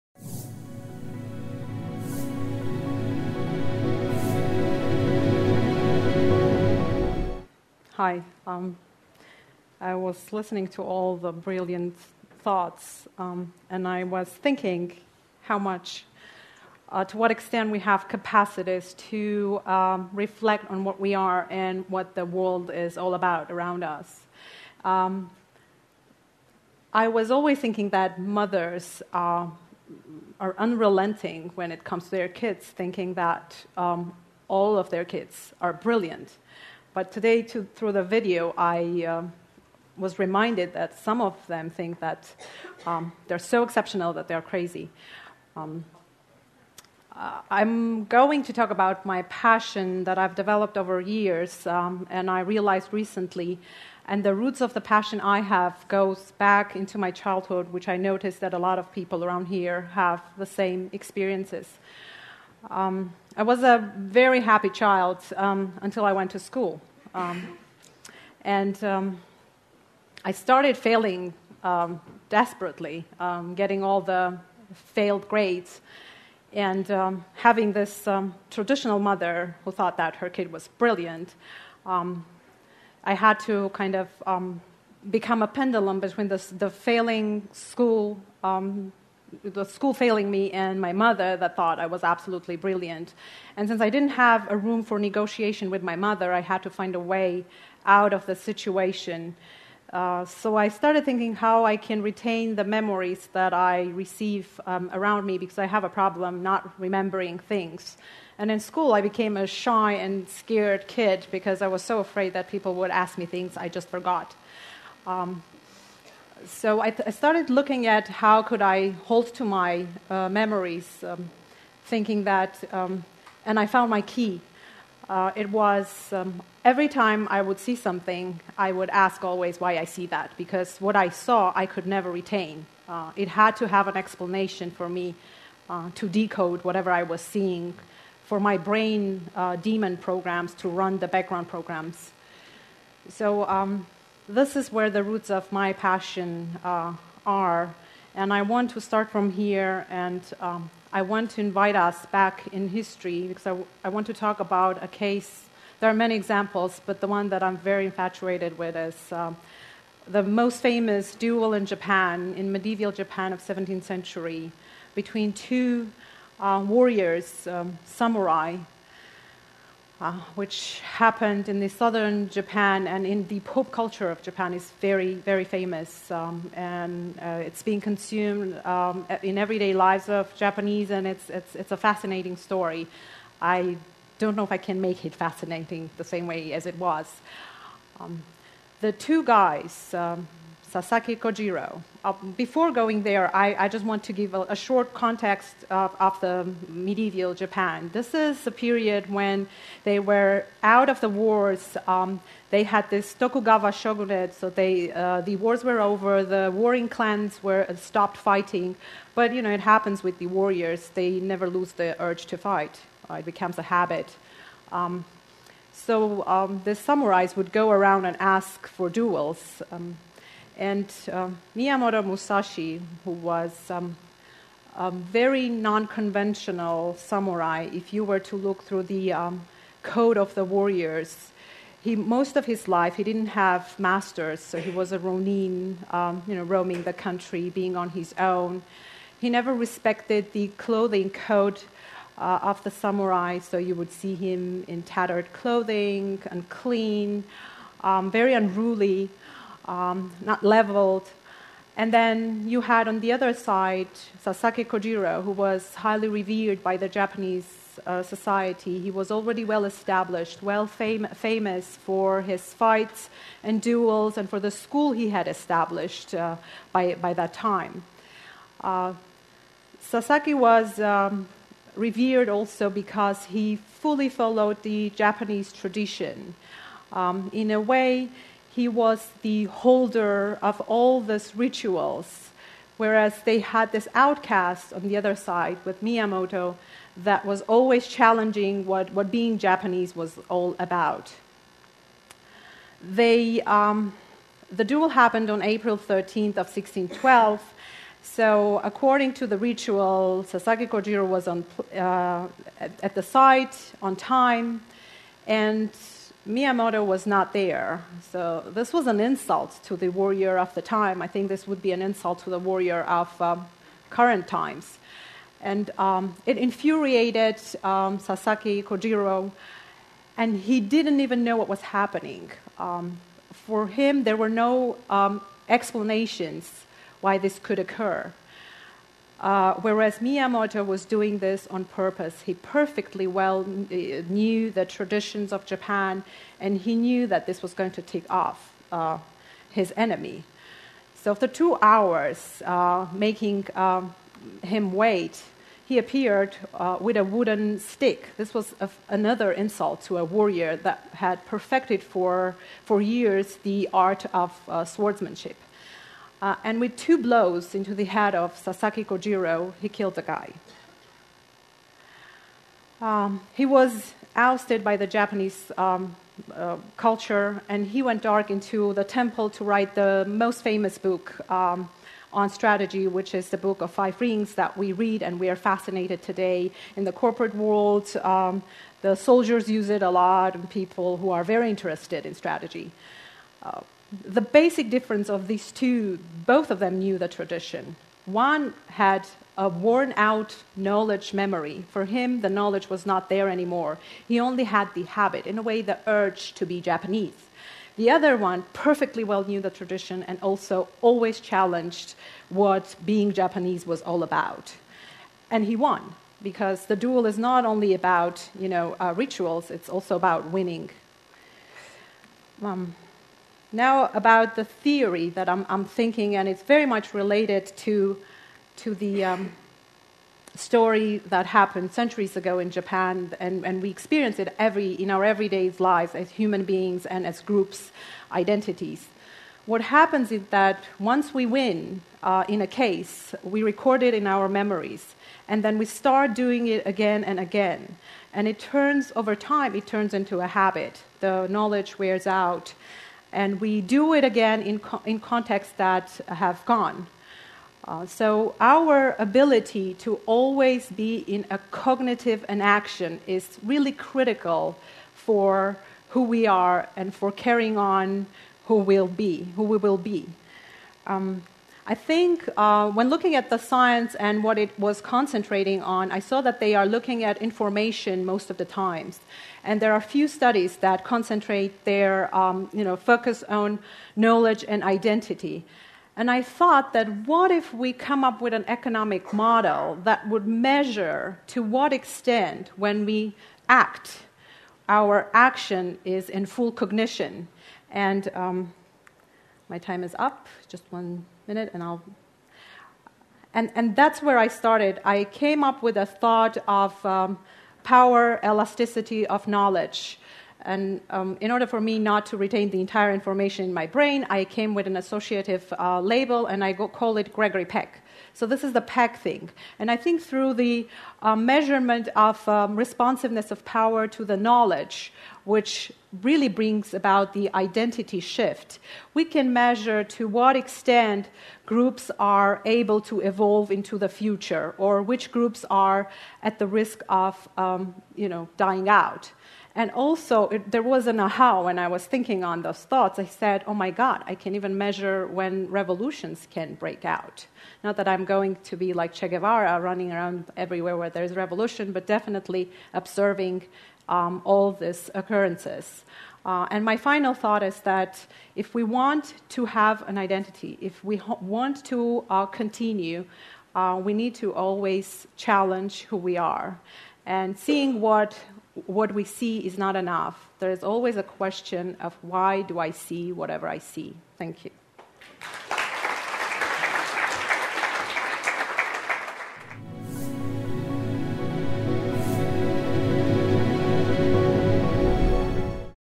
TEDxYerevan - Nvard Manassian - Question YourSELF If You Want to Have One
Nvard Manassian teaches at the Bryusov Linguistic University of Yerevan. She has worked for both governmental agencies and non-governmental organizations in the field of education. As her career has evolved, she has come to believe that whatever we consider to be "sound" education is geared towards perfecting knowledge of the past. She prefers to focus on making sense of new knowledge and transform the self.